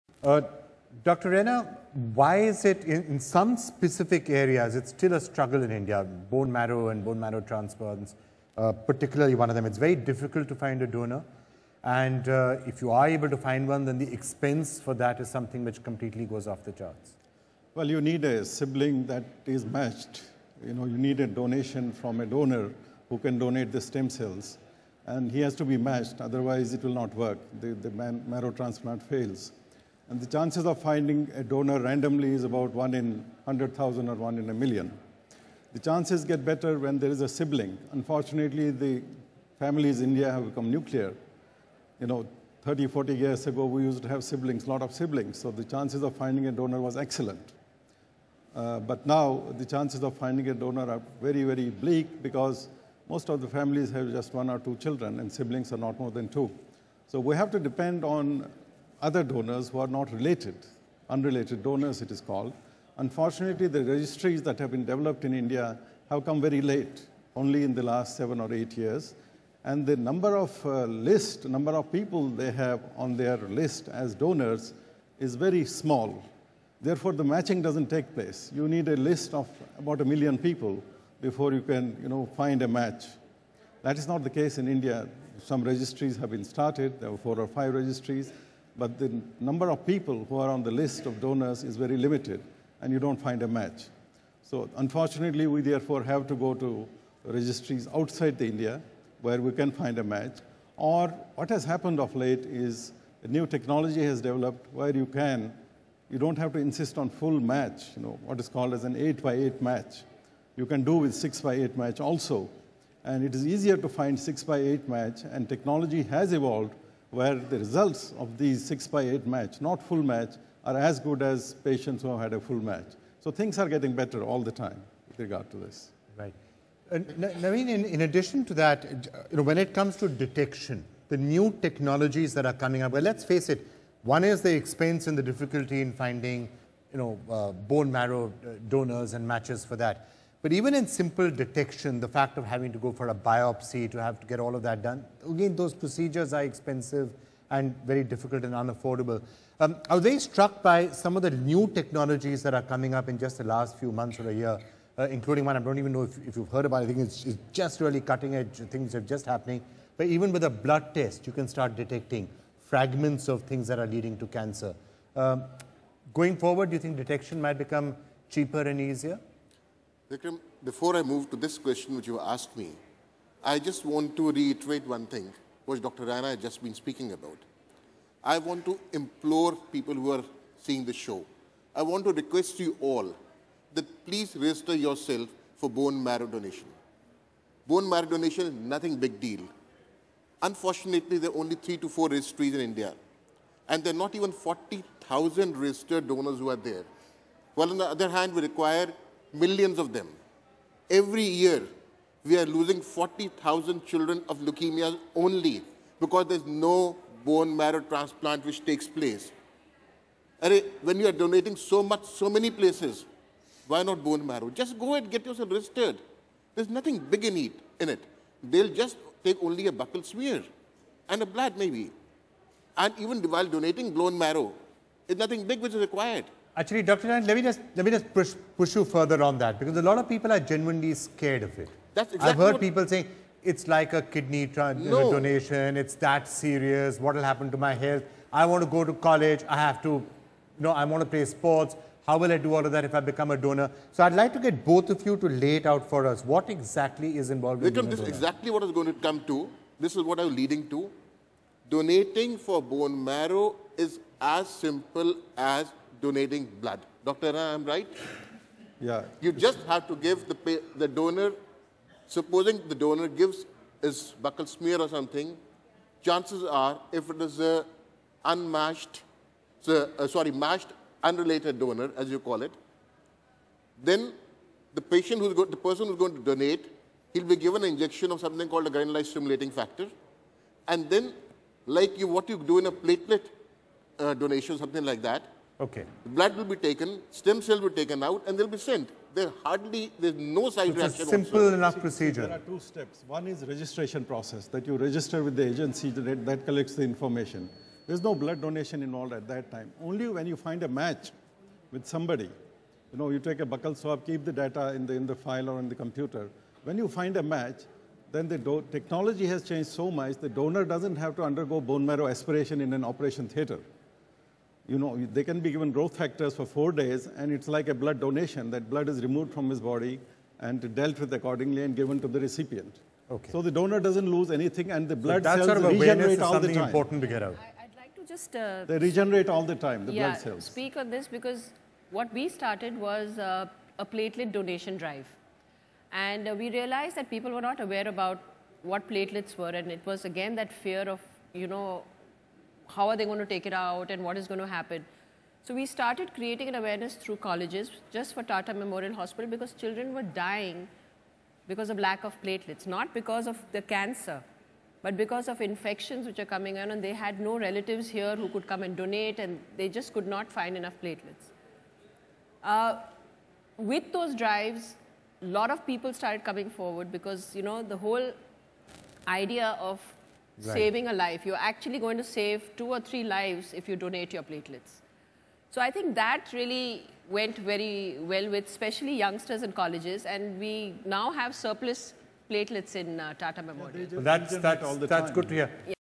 India needs more bone marrow donors
During the NDTV-Fortis Cancerthon, Dr Vinod Raina, Director & HOD - Medical Oncology, Hematology & BMT in Fortis Memorial Research Institute and Dr. Naveen Dang, Leading Microbiologist highlighted the urgent need for Indians to donate bone marrow. There are just a few thousand registered bone marrow donors in India. This is when the demand is for millions.